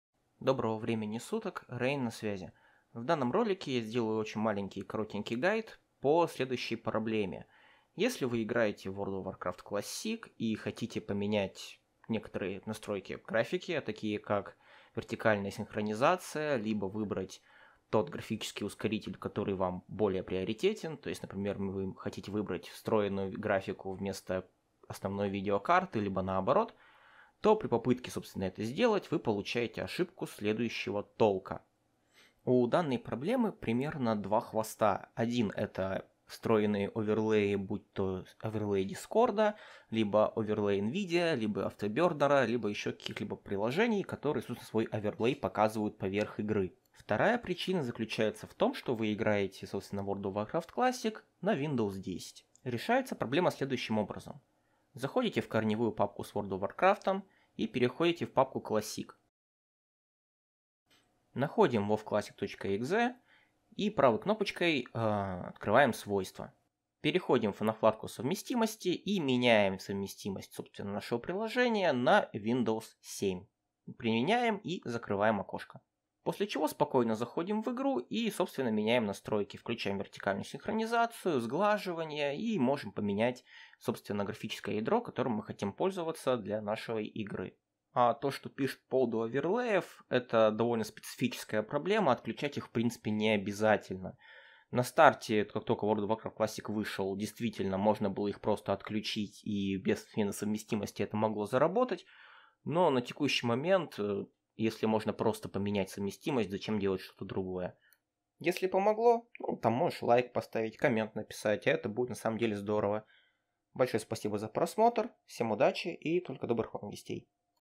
Не удалось инициализировать ускоритель трёхмерной графики / Unable to start up 3D Acceleration (FIX)
Решение проблемы с WoW Classic при включении вертикальной синхронизации и некоторых настроек графики
A universal solution to the problem with the World of Warcraft classic - Unable to start up 3D Acceleration